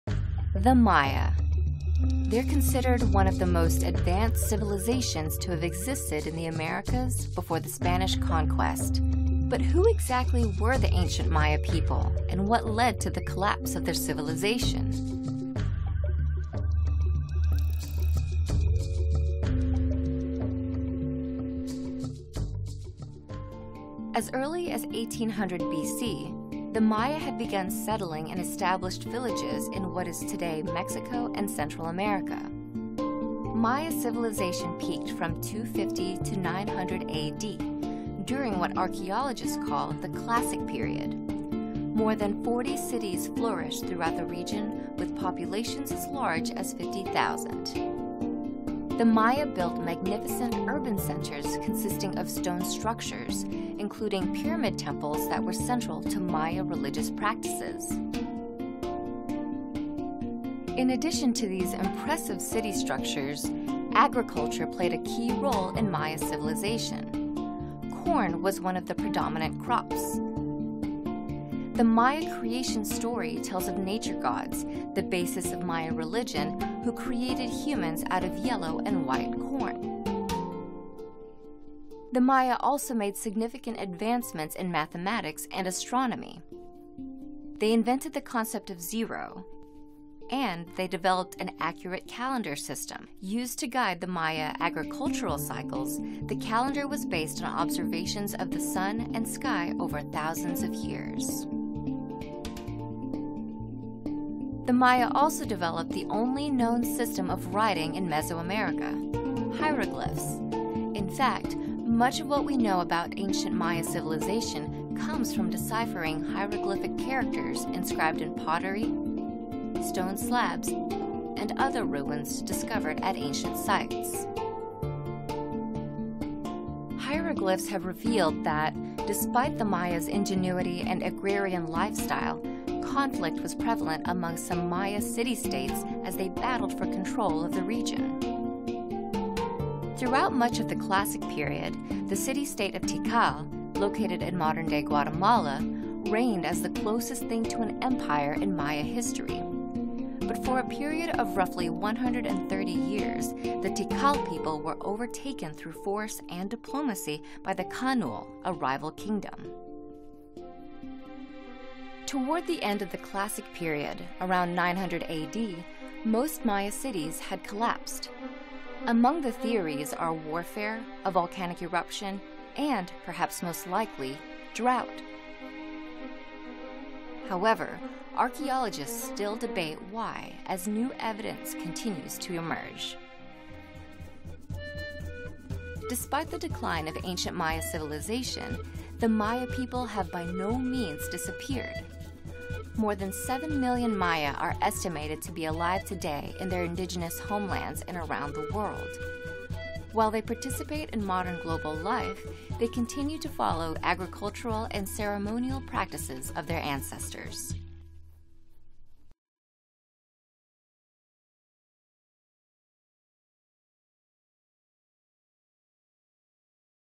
Ancient Maya Civilization
With their impressive city structures and advanced astronomical understanding, the Maya civilization once dominated Mesoamerica. Learn about the Maya's influence in mathematics, how their cosmic calendars advised agricultural matters, and how the legacy of this ancient civilization endures through Maya people today.